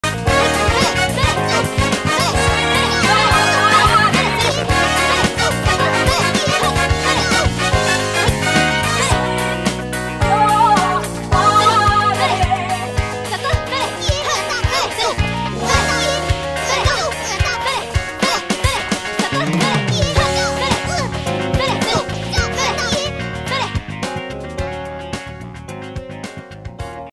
Maori Infinite
This is Maori's actual infinite.  32 hits is my personal best